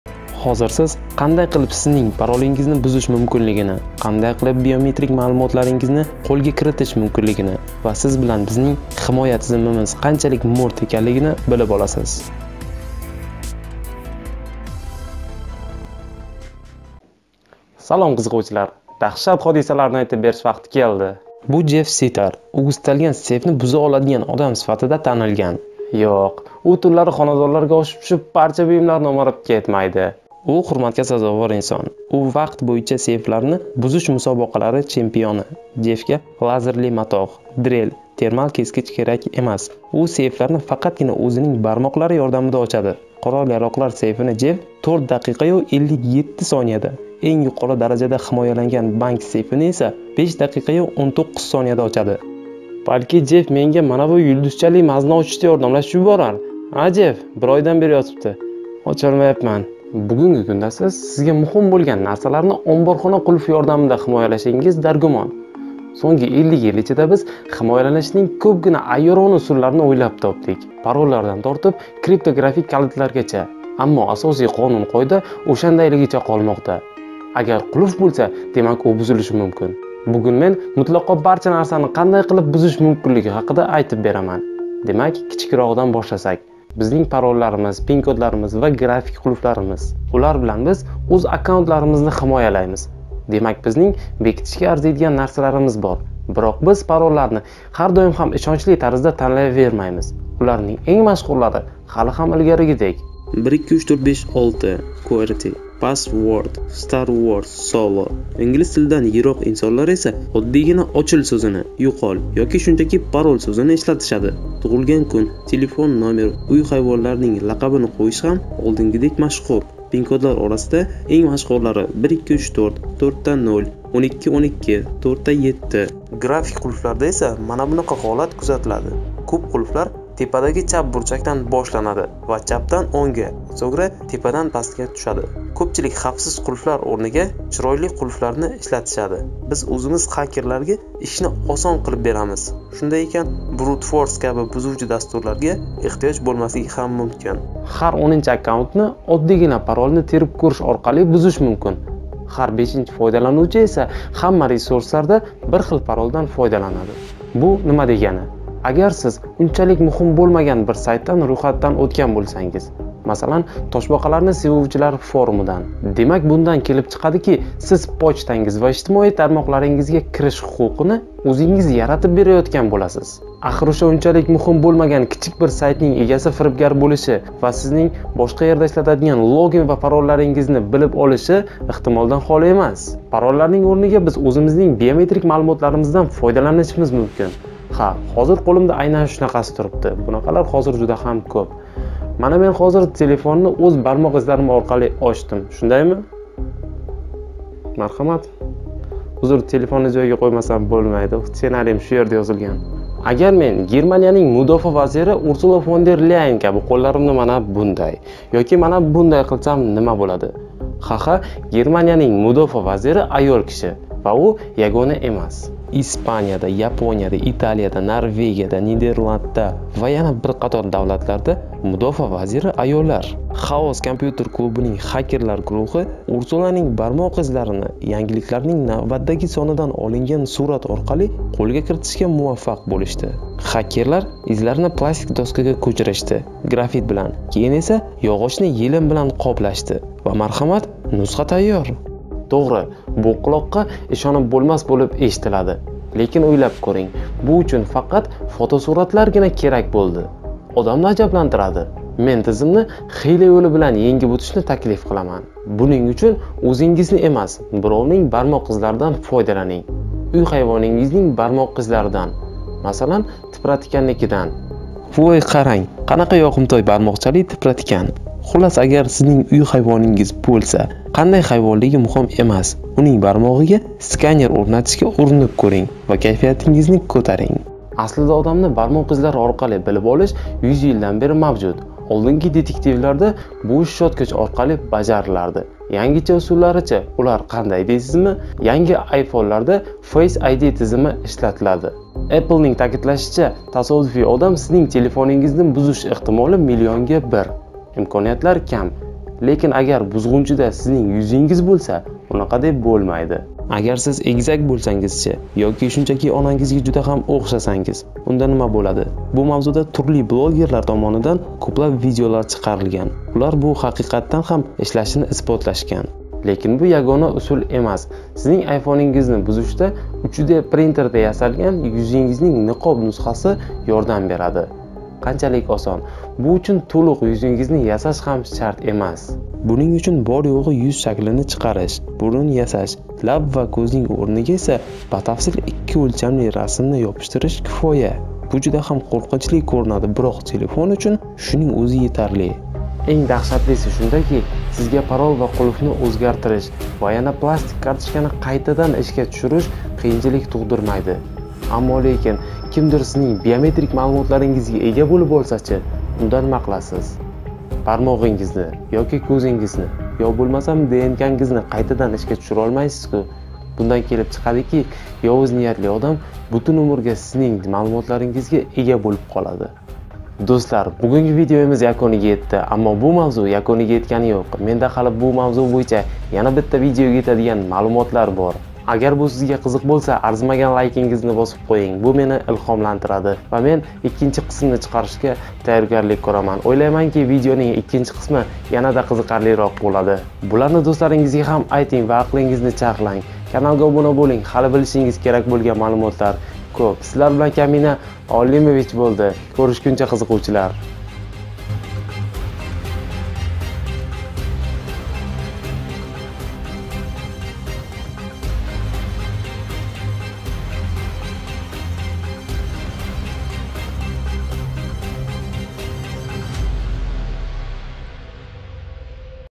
ПАРОЛИНГИЗНИ 5 ДАКИКАДА ТОПАМАН — БУ КИЗИК
Hozir siz qanday qilib sizning parolingizni buzish mumkinligini, qanday qilib sizning beometrik ma’lumotlaringizni qo’lga kiritish mumkinligini, va, siz bilan bizning himoya tizimimiz qanchalik mo’rt ekanligini bilib olasiz.

Qanday qilib tizimni buzish mumkin? (1-qism)
Қандай қилиб тизимни бузиш мумкин? (1-қисм)